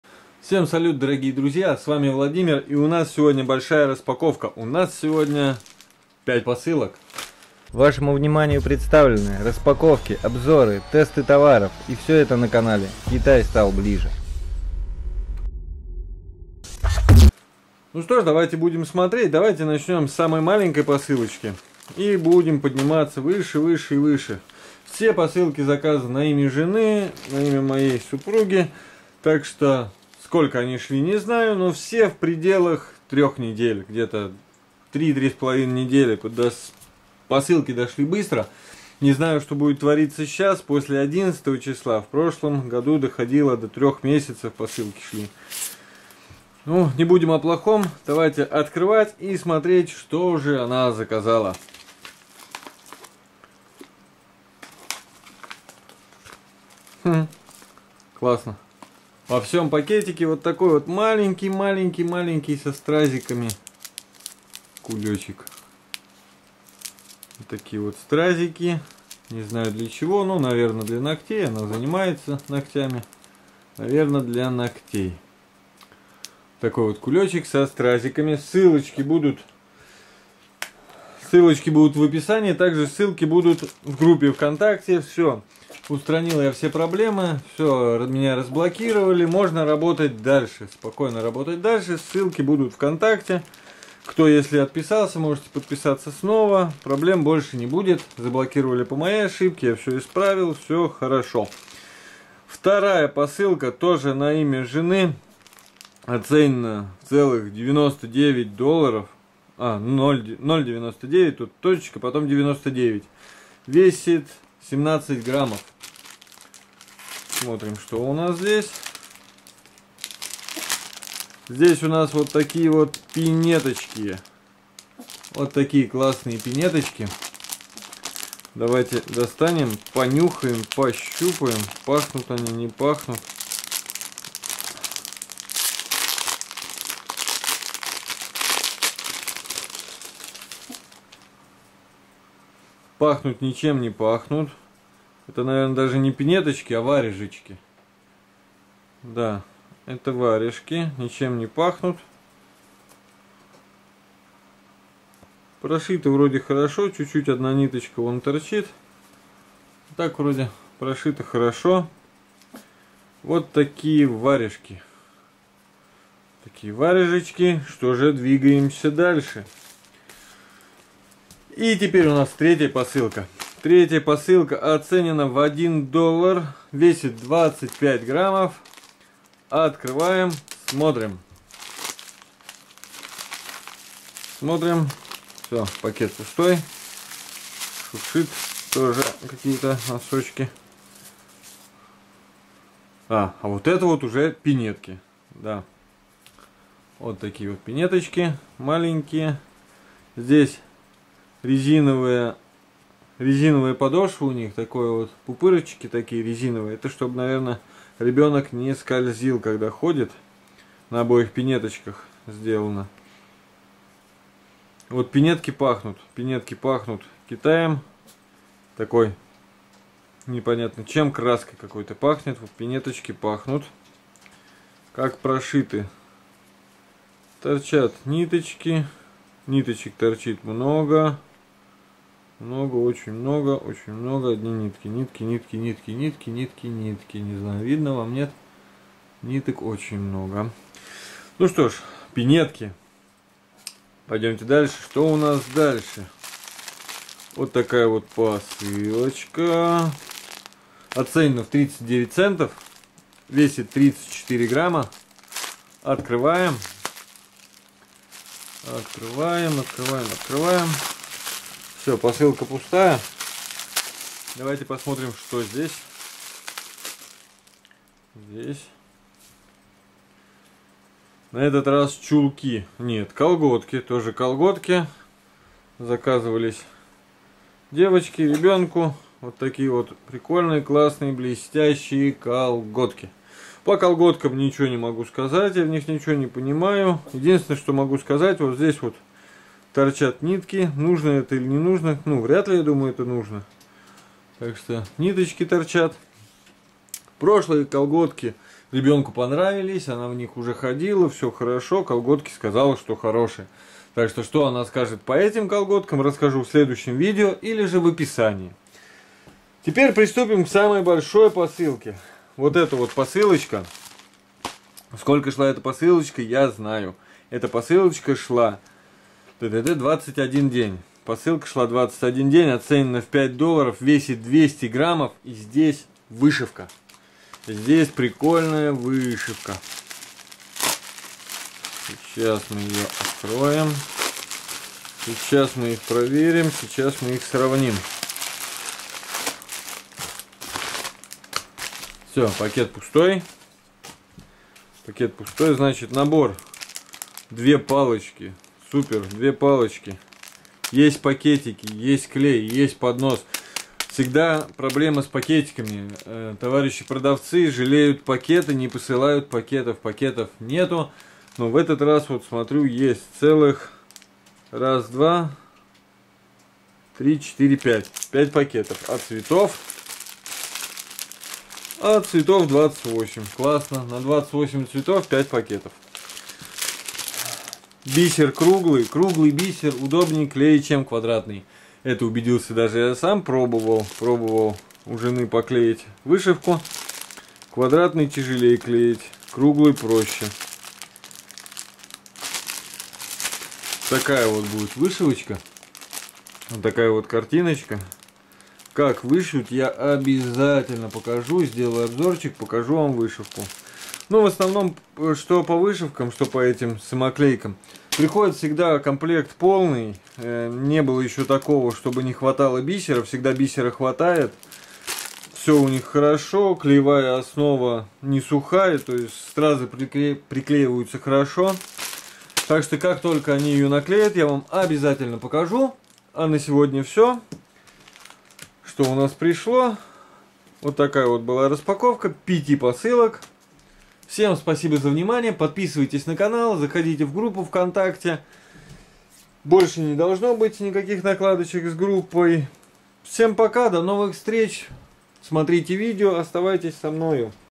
Пять посылок. Всякая всячина для всей семьи
Всем привет, сегодня пришла куча посылок.

Помощь каналу:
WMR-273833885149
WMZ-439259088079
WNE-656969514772